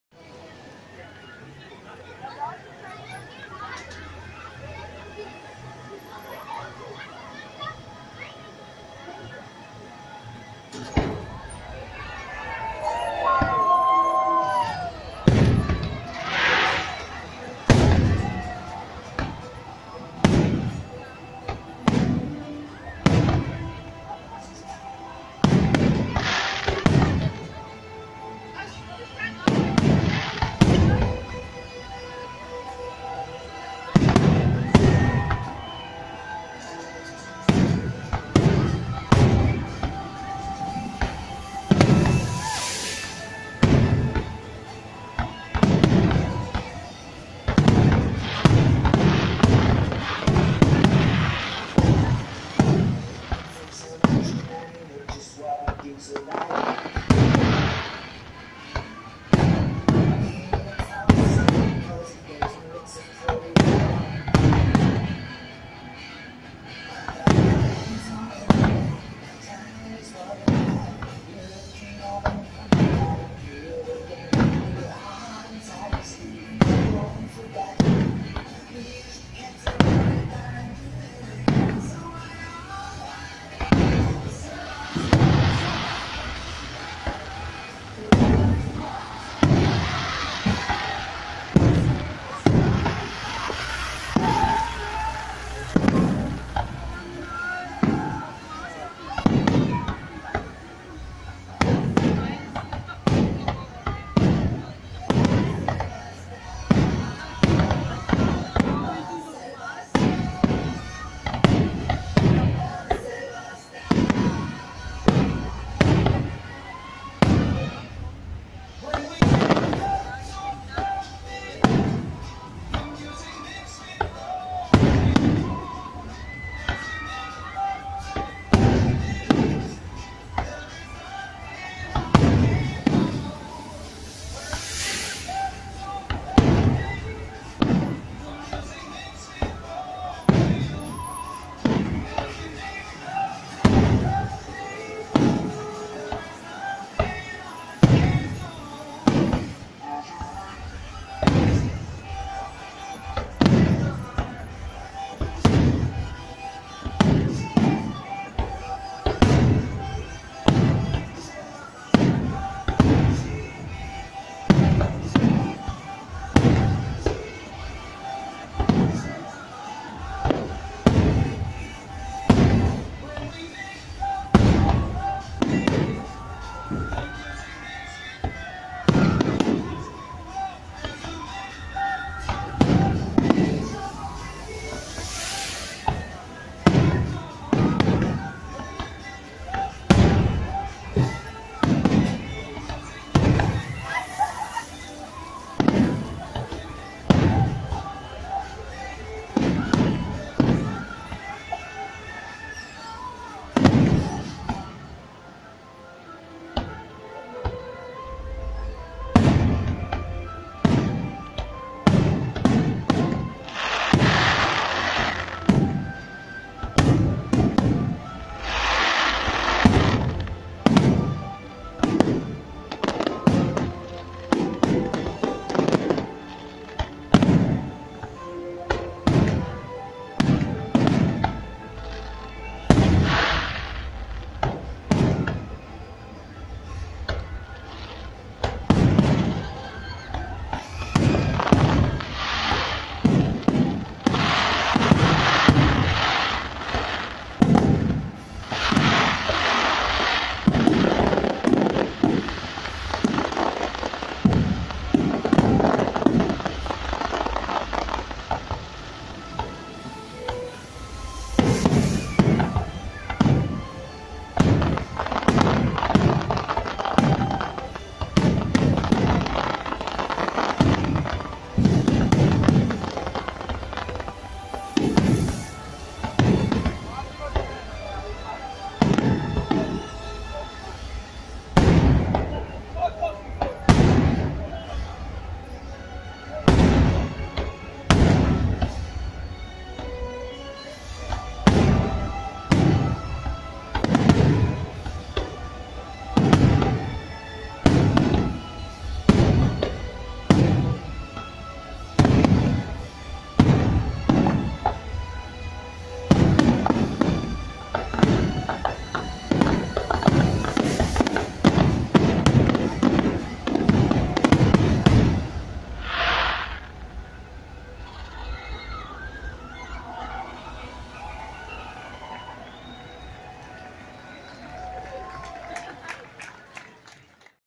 EUROPAPARK | FEU D'ARTIFICE DE LA NOCTURNE 2017
Feu d'artifice du 22/07/2017 d'Europapark, filmé depuis la splashzone d'Atlantica.